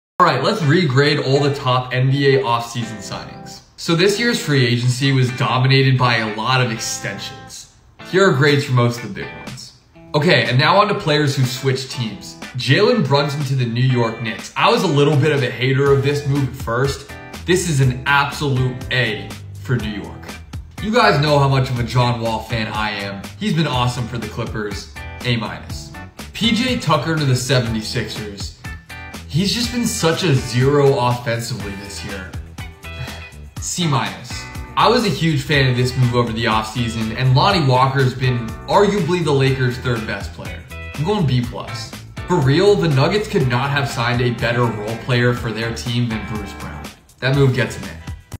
Re-grading the NBA's BIGGEST Offseason Signings!
handing out grades after we've seen each of the NBA's biggest offseason signings – Jalen Brunson, John Wall, PJ Tucker, Lonnie Walker, etc – in action for a few weeks.